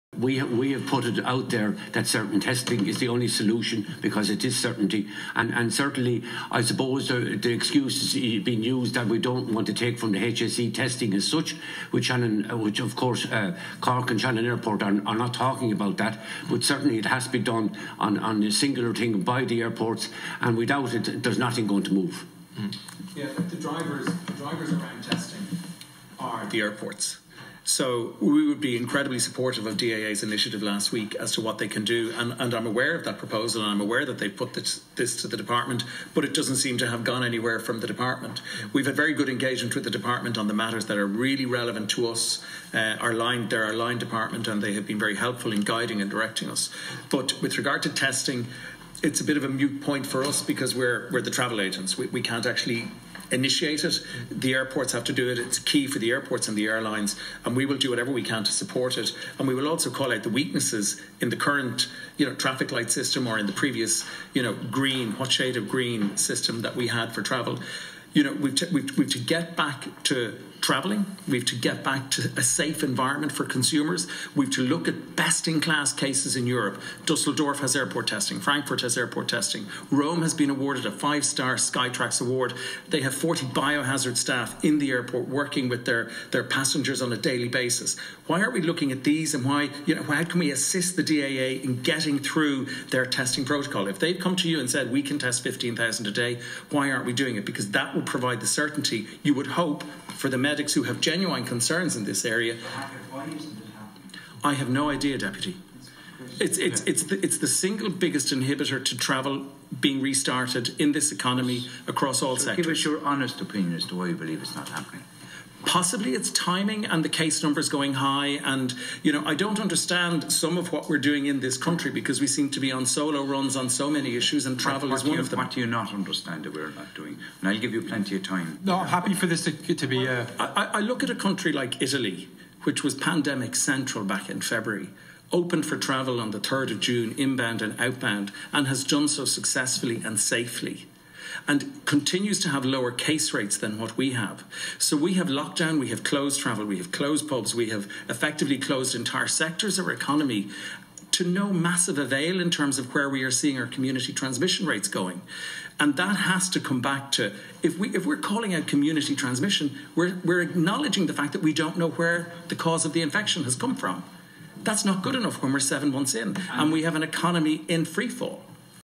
Testing is the only solution says Paul Hackett
Paul Hackett Board Member of the ITAA
Evidence to the Oireachtas Transport Committee on the impact of Covid-19, October 14 2020